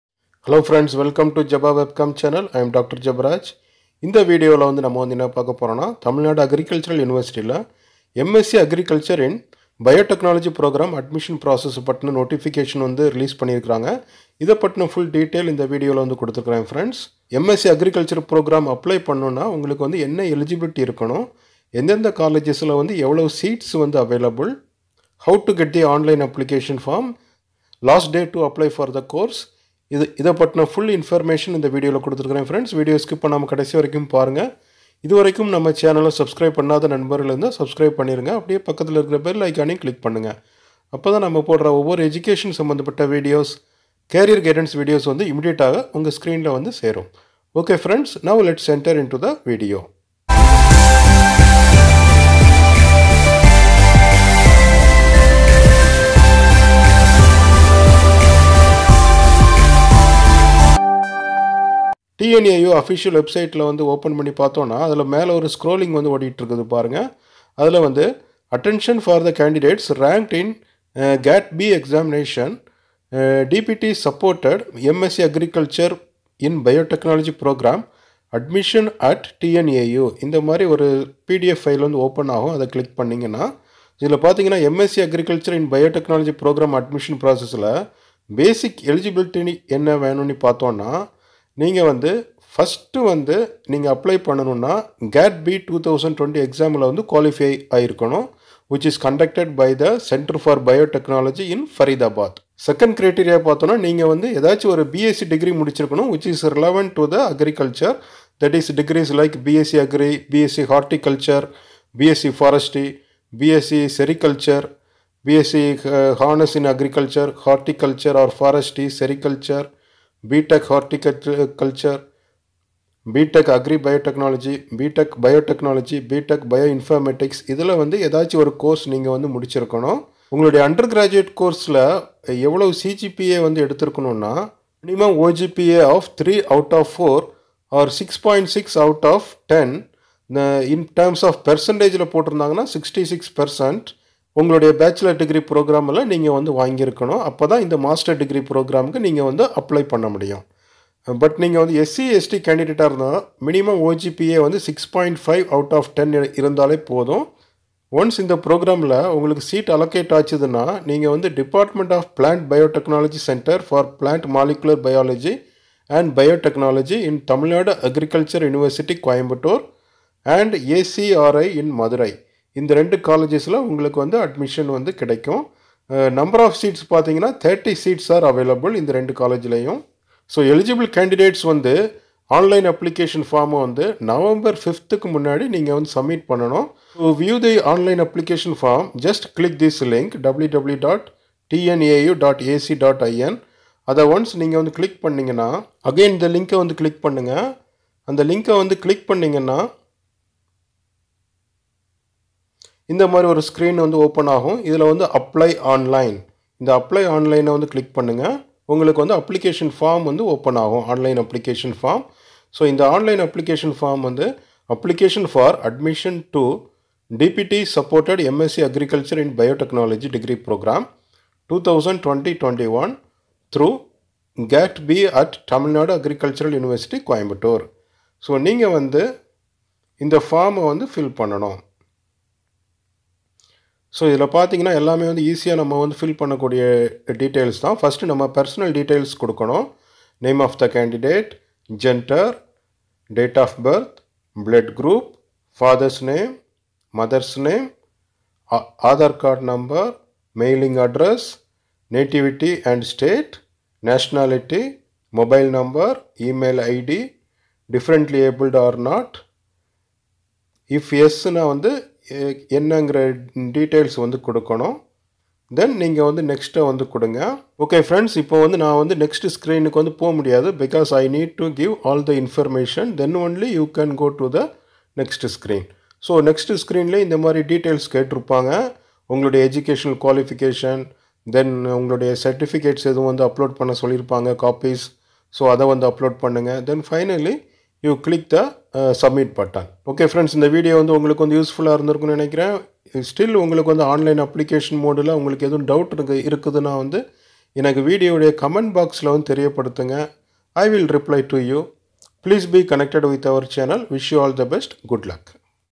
TNAU - MSc (Agriculture) in Bio-Technology Admissions Update | Full Details in Tamil
In this video,  the complete details about the MSc (Agriculture) in Bio-Technology Admissions in TNAU is explained in Tamil language.

Jeba Webcam Youtube channel has been created by Dr.Jebaraj S., M.E., Ph.D. with a vision of publishing videos in career guidance & education.